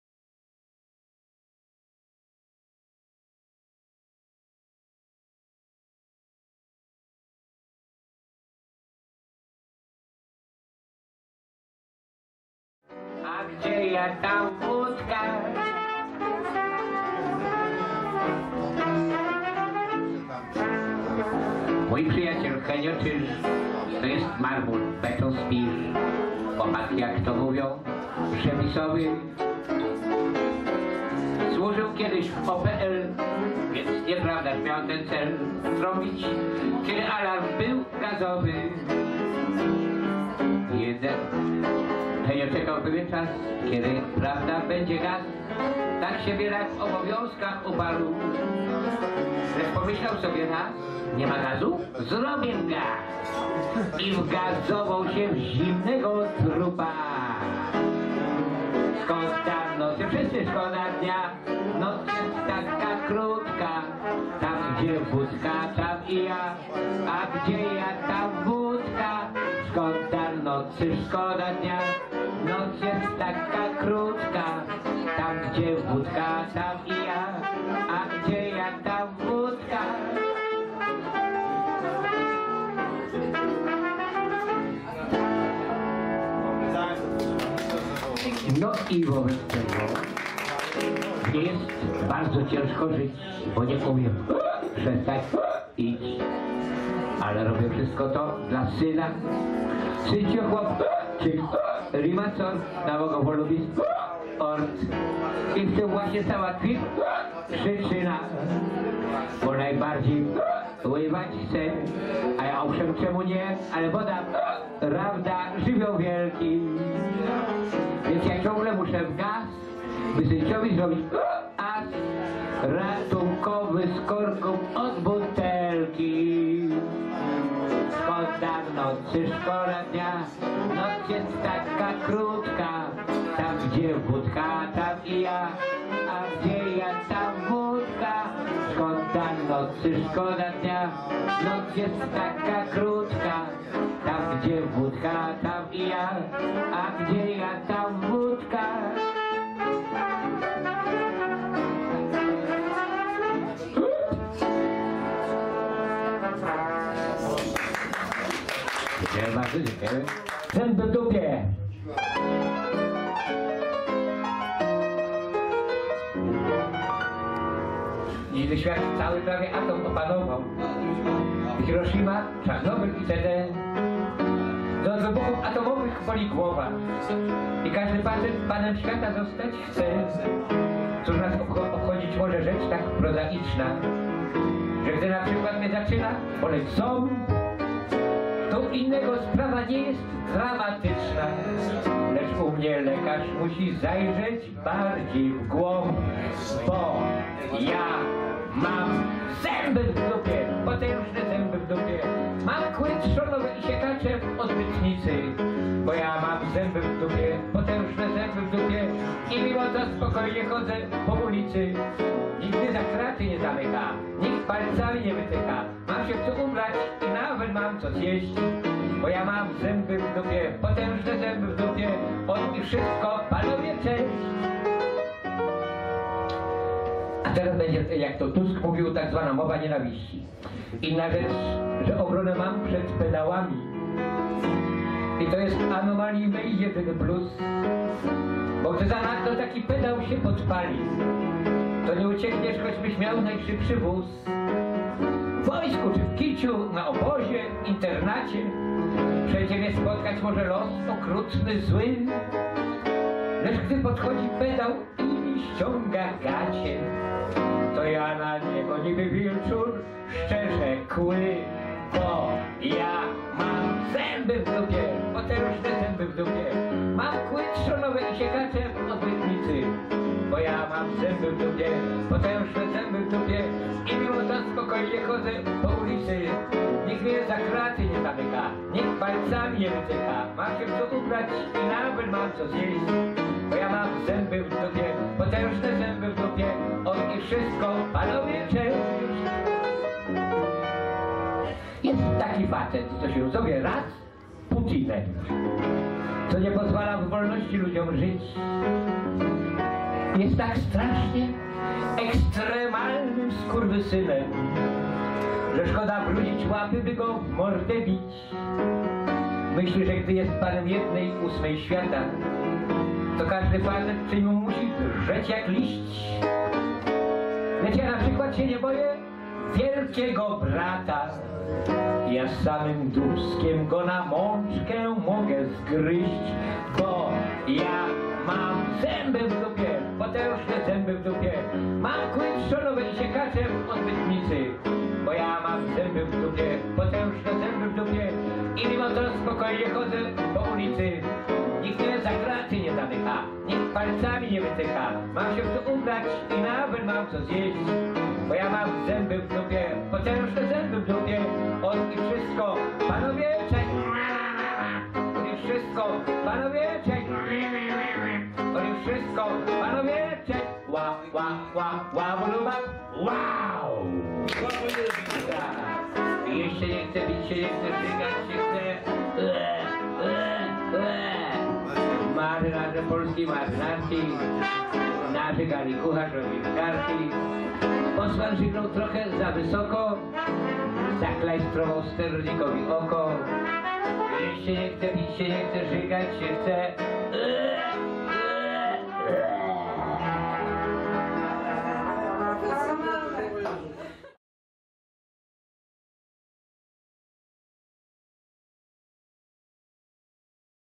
JUREK BOŻYK W PUBIE AWARIA - 22.03.2015
Oto fragment recitalu jednego ze sztandarowych artystów krakowskiej bohemy muzycznej. :) Zawsze w formie,pełen wigoru i oryginalnych kompozycji muzycznych. :)